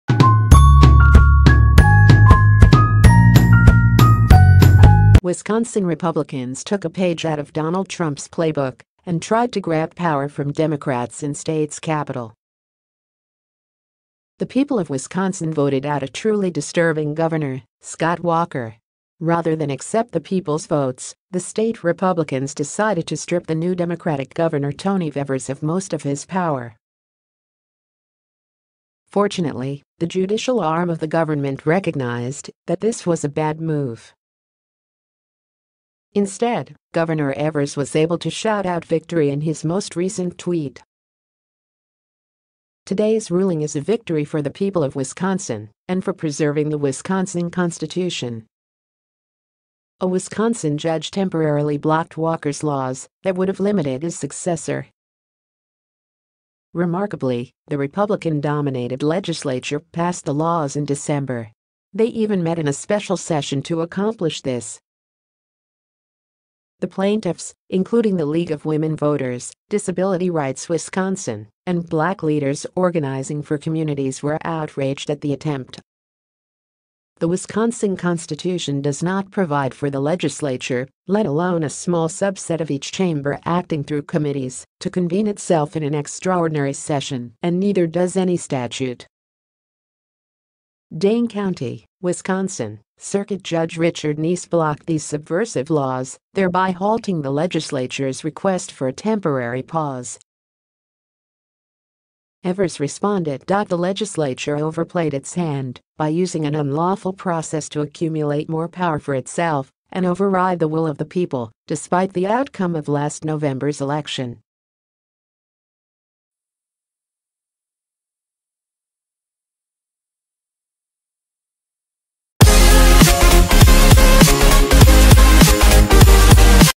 Wisconsin Republicans took a page out of Donald Trump’s playbook
Wisconsin Republicans took a page out of Donald Trump’s playbook and tried to grab power from Democrats in state’s capitol.  The people of Wisconsin v